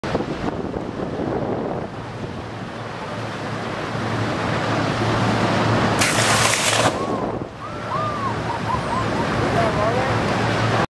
Dolphins in Loreto Mexico
Bottle nose dolphins racing with our boat in Loreto Mexico